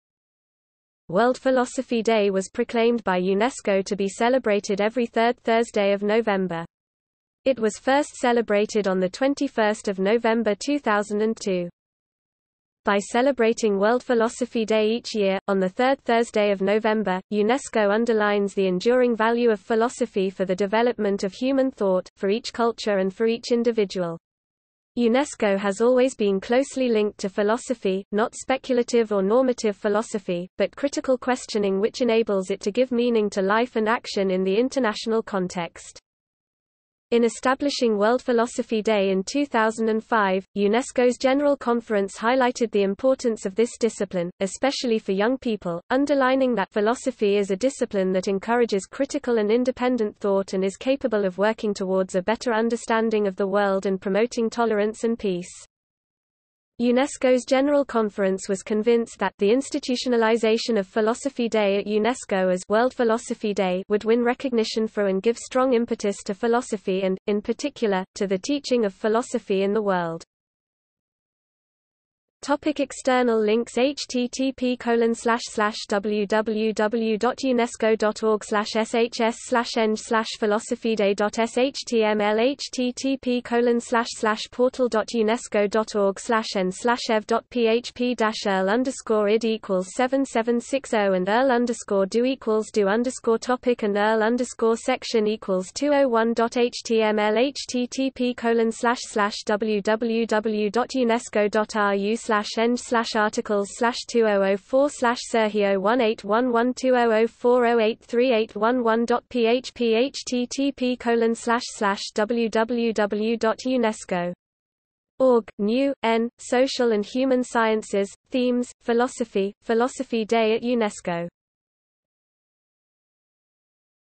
World Philosophy Day | Wikipedia audio article
00:01:12 External links

- Socrates

SUMMARY
World Philosophy Day was proclaimed by UNESCO to be celebrated every 3rd Thursday of November. It was first celebrated on 21 November 2002.
By celebrating World Philosophy Day each year, on the third Thursday of November, UNESCO underlines the enduring value of philosophy for the development of human thought, for each culture and for each individual. UNESCO has always been closely linked to philosophy, not speculative or normative philosophy, but critical questioning which enables it to give meaning to life and action in the international context.
In establishing World Philosophy Day in 2005, UNESCO’s General Conference highlighted the importance of this discipline, especially for young people, underlining that “philosophy is a discipline that encourages critical and independent thought and is capable of working towards a better understanding of the world and promoting tolerance and peace”.
UNESCO's General Conference was convinced that “the institutionalisation of Philosophy Day at UNESCO as ‘World Philosophy Day’ would win recognition for and give strong impetus to philosophy and, in particular, to the teaching of philosophy in the world”.